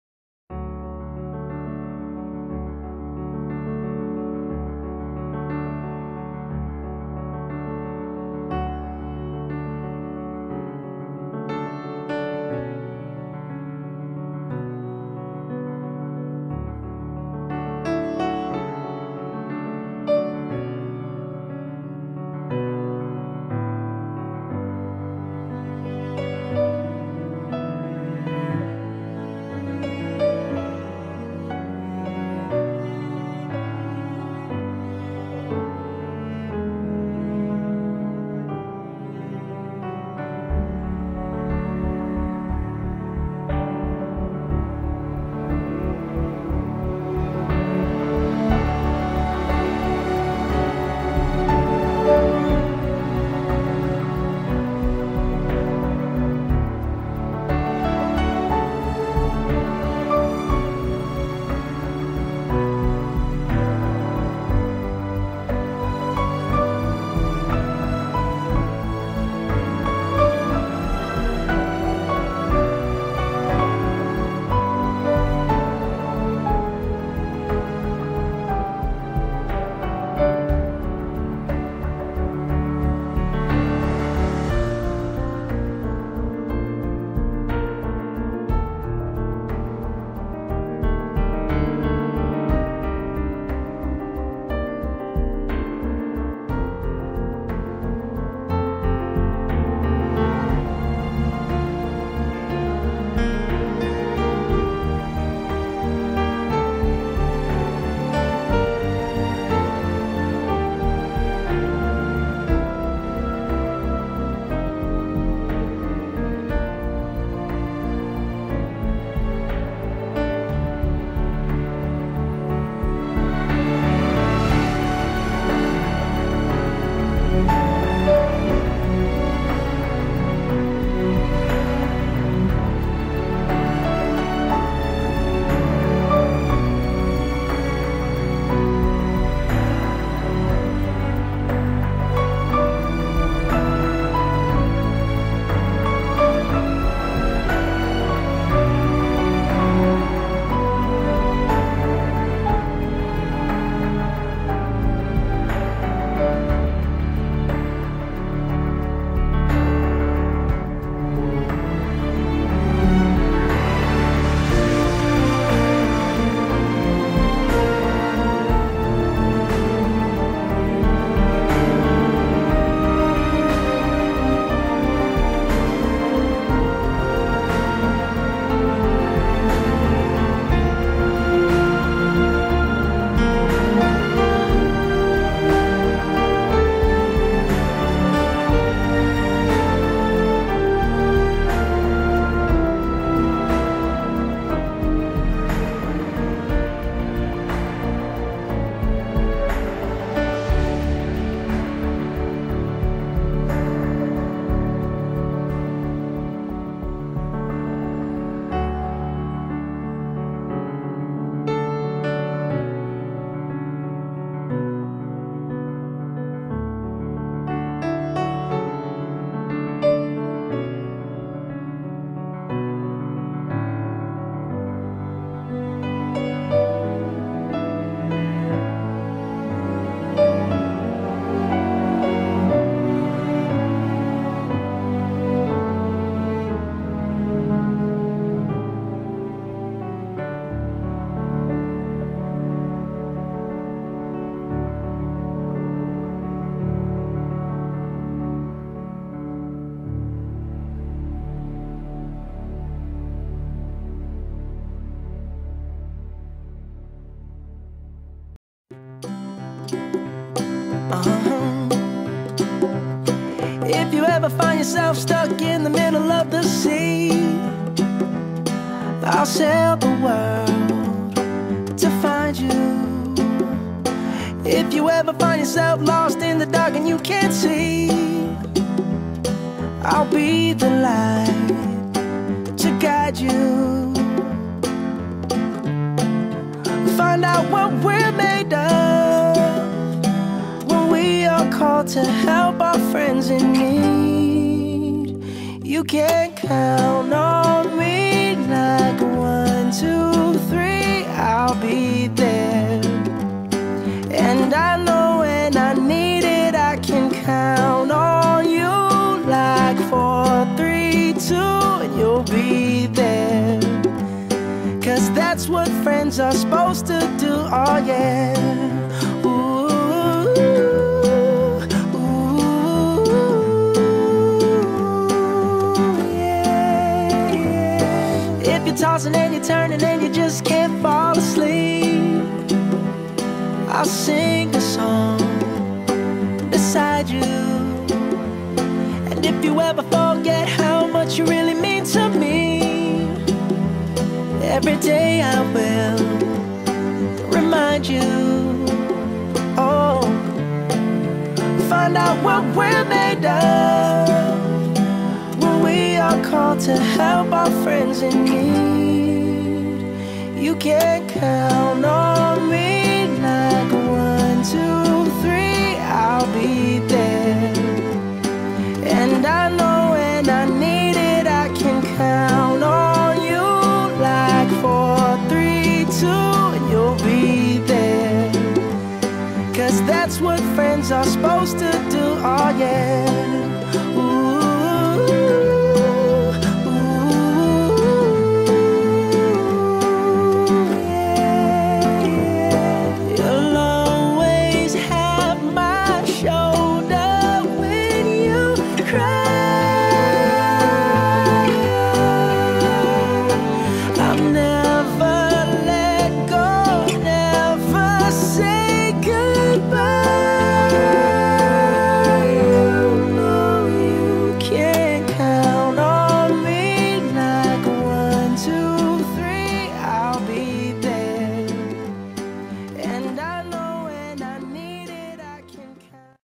The life of a showdog; after the show!
A look at the life of a show dog when they aren't in the ring!